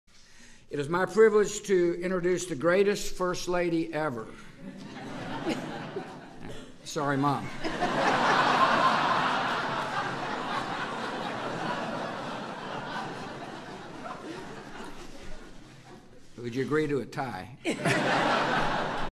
George W. Bush Jokes About Who was the Best First Lady (2012)
Source:

Obama White House